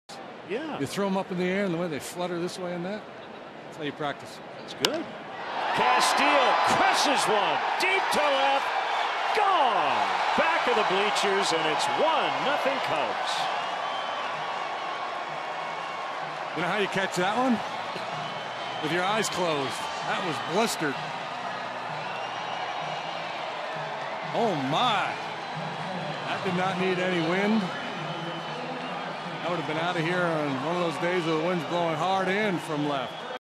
PHI@CHC: Castillo crushes one to left
4/4/14: Welington Castillo crushes a home run over the left-field wall as the Cubs take the lead